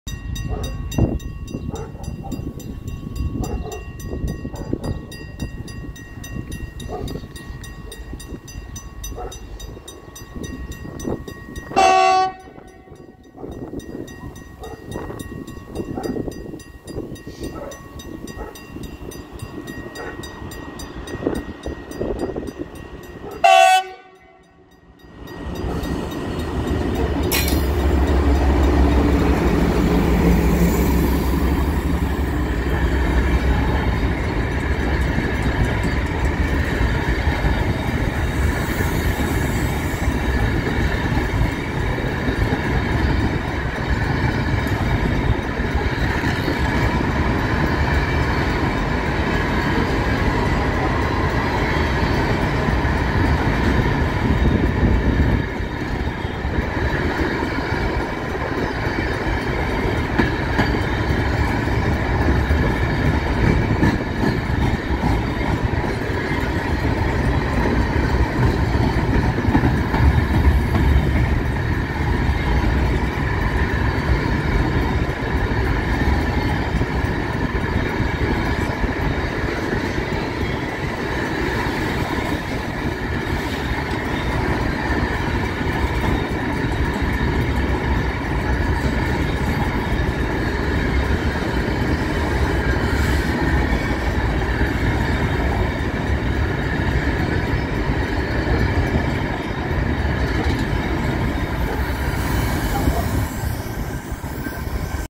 Trenes Argentinos Cargas por Estación San Nicolás
Trenes Argentinos Cargas 9423 por San Nicolás con 38 tolvas, con dirección  norte. Vídeo del viernes 29 de septiembre  a las 16:20